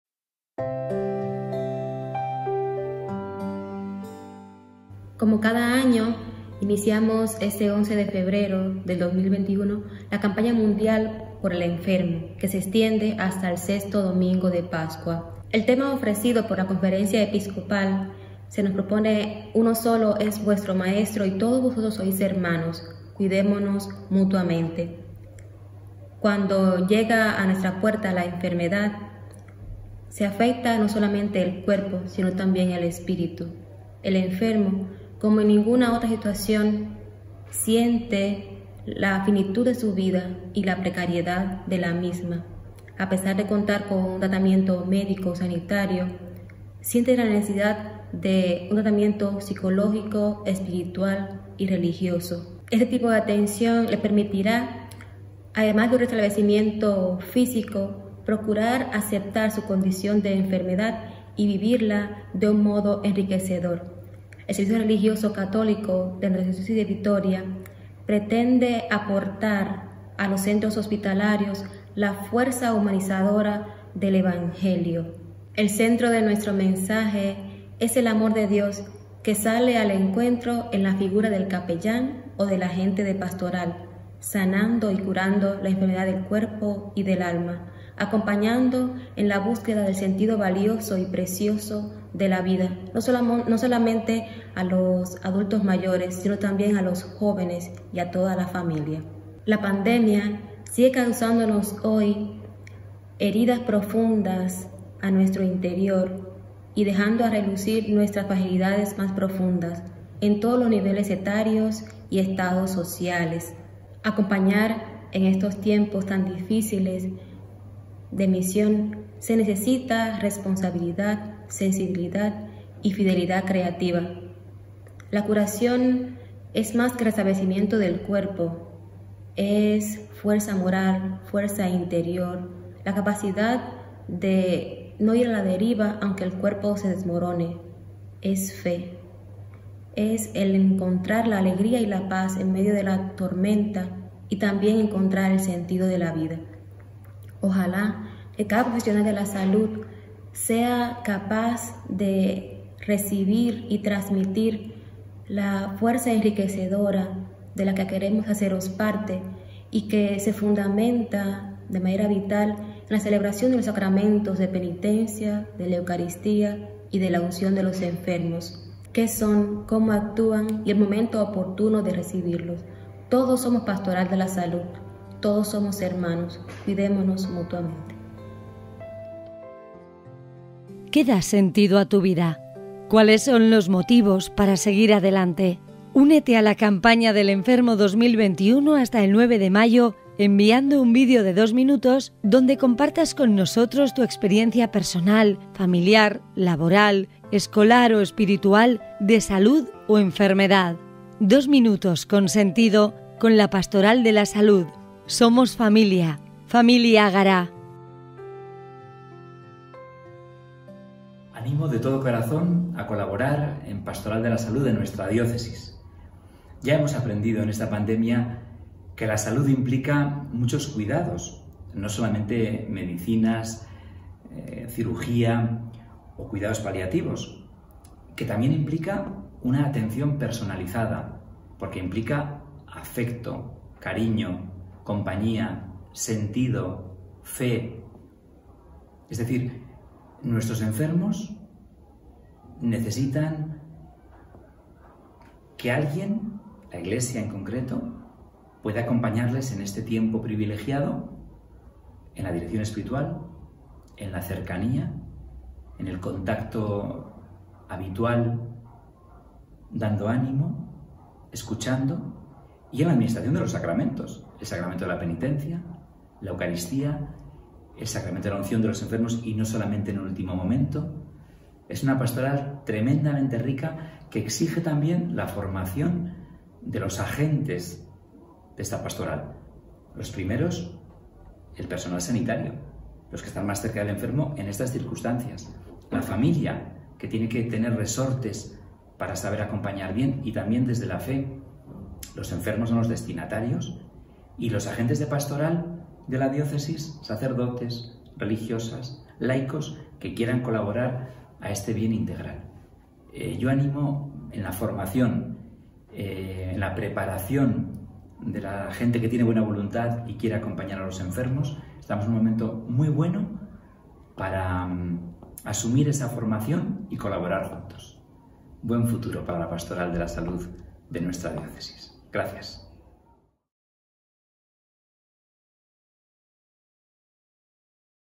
XXIX Jornada Mundial del Enfermo
El 11 de febrero, festividad de Nuestra Señora de Lourdes, celebramos la Jornada Mundial del Enfermo bajo el lema “Cuidémonos mutuamente”. En este vídeo la Delegada de Pastoral de la Salud, Sor Daniuska Rodriguez, y el Obispo de Vitoria, D. Juan Carlos Elizalde, tienen un mensaje para ti.